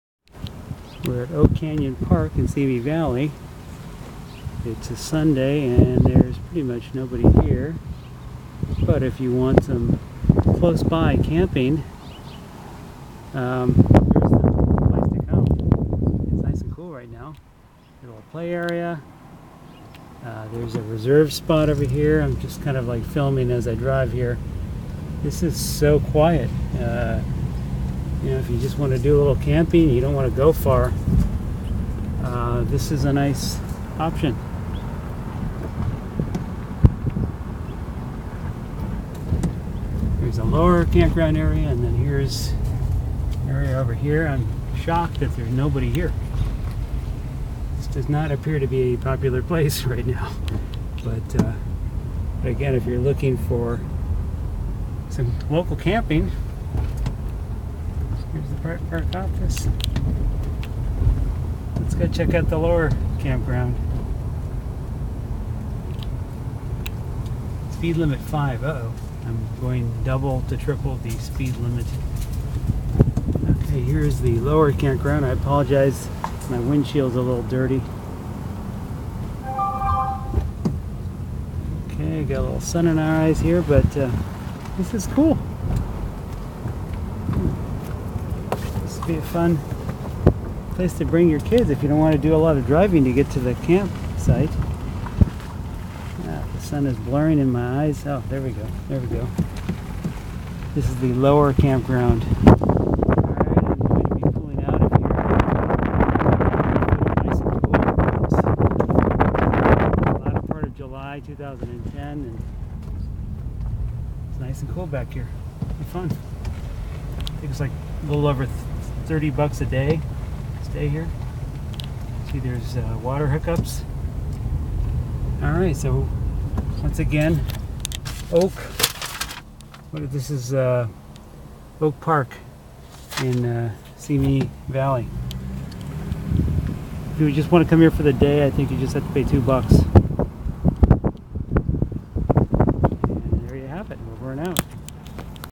Oak Park Campground in Simi Valley
When I drive the 118 Freeway from Thousand Oaks via the 23 into Simi Valley, I always notice a sign on the freeway that says "Oak Park Campground" but never took the time to check it out. Well I did yesterday and found what was pretty much a completely empty campground on a Sunday evening.  This is a 100 acre open space park with RV hookups, plenty of space to roam around, playgrounds, horseshoes, etc. Quite peaceful actually. Immediately it dawned on me that this would be a great little quick camping escape with my little guys, just minutes from home.

For camping and RV options throughout Ventura County and adjacent areas,

For thousands of things to do throughout Ventura County and surrounding areas,